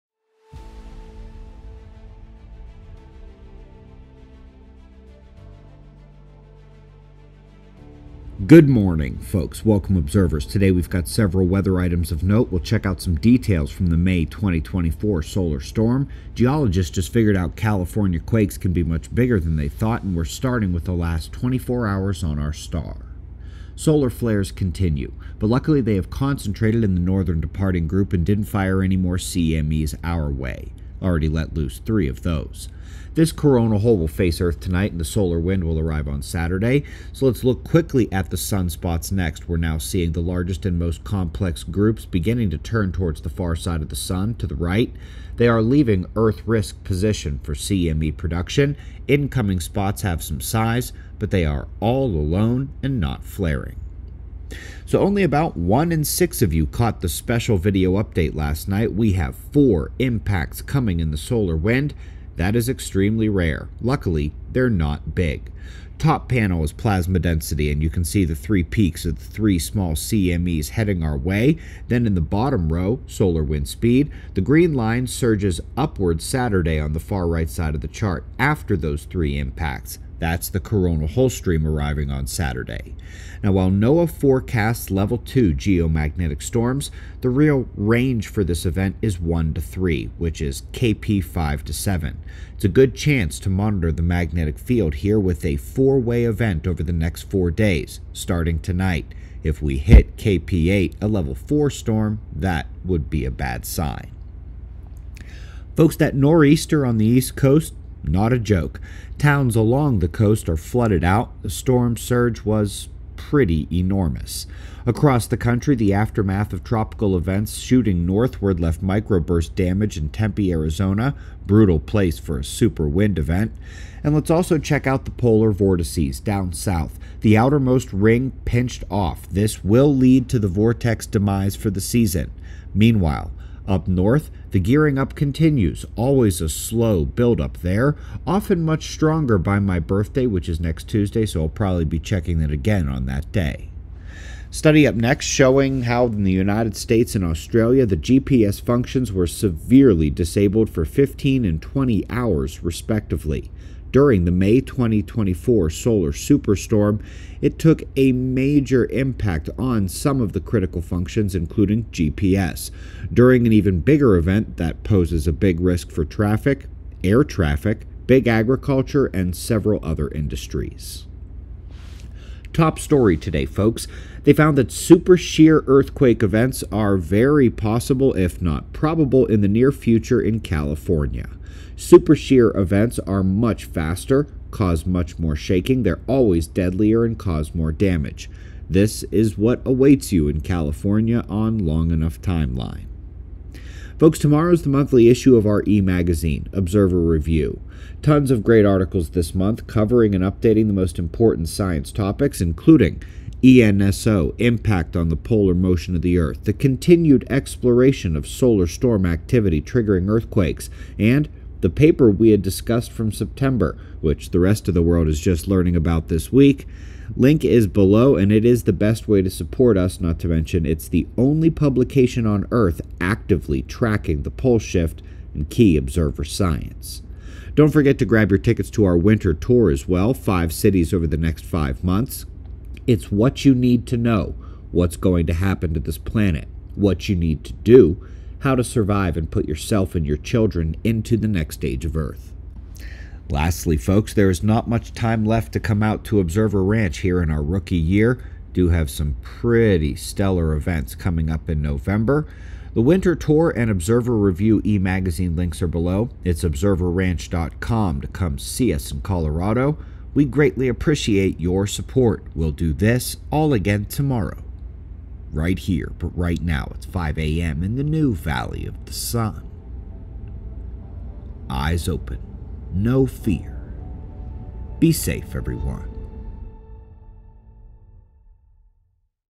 4-Way Solar Storm Coming, California Megaquake Forecast | S0 News Oct.15.2025
Book Pre-Orders Are Still In Progress!!! We are working as quickly as possible to get them out!